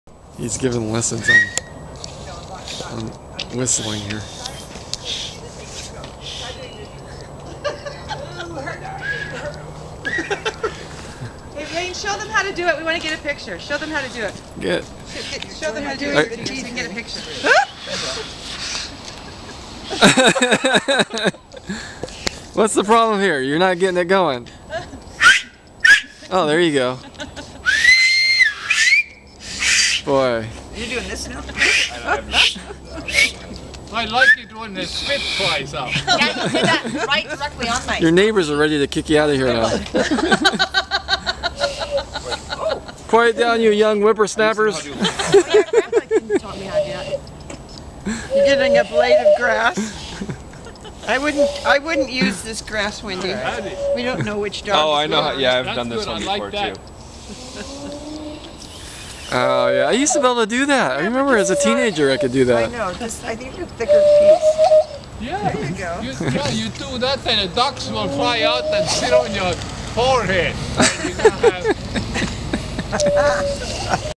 Learning to whistle
Cuz Wendy, Romy, Jeff, Delores, Dad learning to whistle